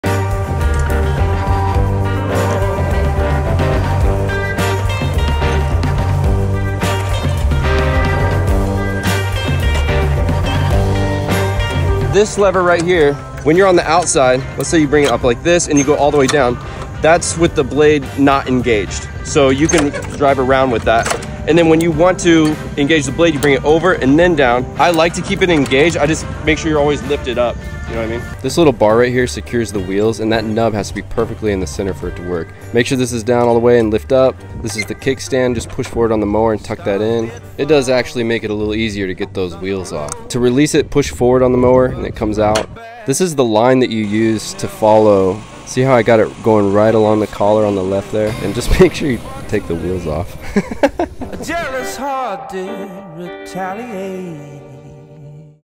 How to Mow Collars | Pt. 3
How to mow bentgrass collars around a golf course green using a Toro Greensmaster Flex reel mower.